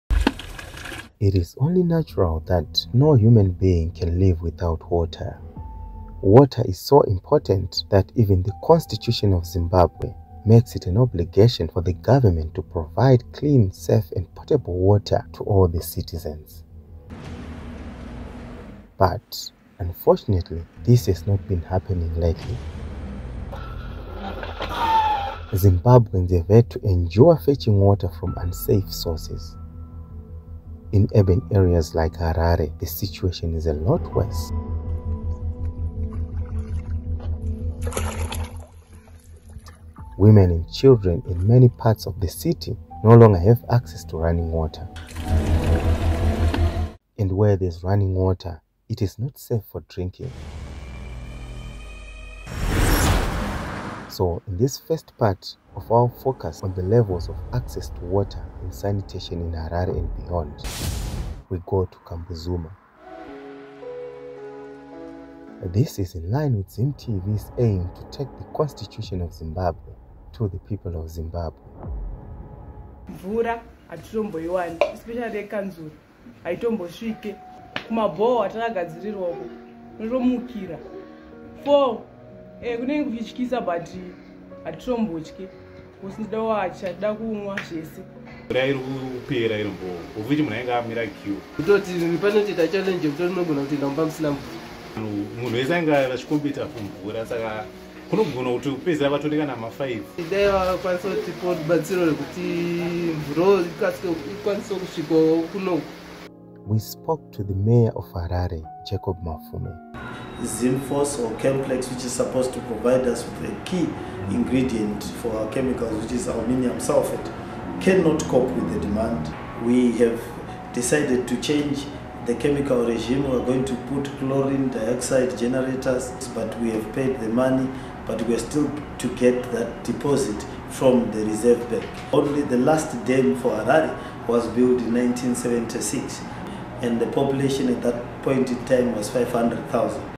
Risking lives to get water in Harare
Are you able to safely access safe, clean and potable water?Or you have to wake up at 4 a.m, risk your life to get water?In this short documentary, we go to Kambuzuma.
Find out what is happening there and what the Mayor of Harare Jacob Mafume has to say.
Also share with us your experiences.